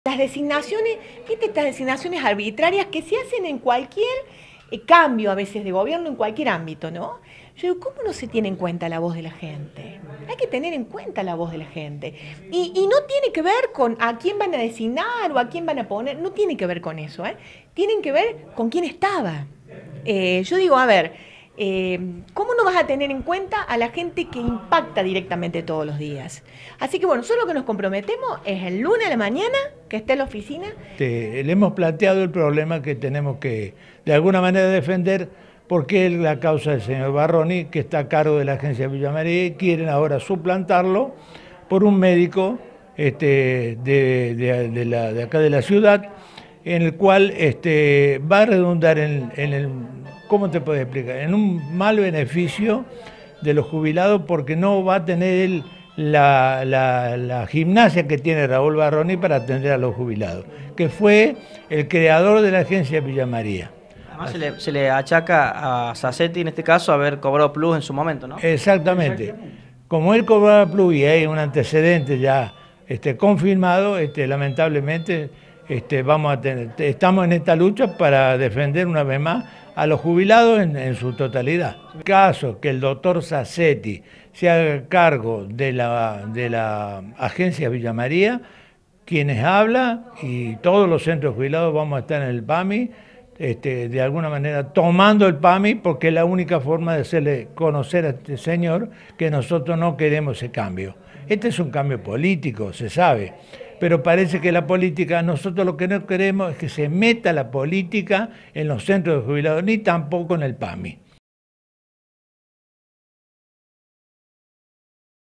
PAMI EN PIE DE GUERRA PIDIERON EN LA AUDITORIA POR VARRONI
Los jubilados de Villa María y la región están en pie de guerra. Quieren que Raúl Varroni continúe como director del PAMI local y cuestionan fuertemente la designación de Juan Zazzetti. Hoy se reunieron con Peresutti.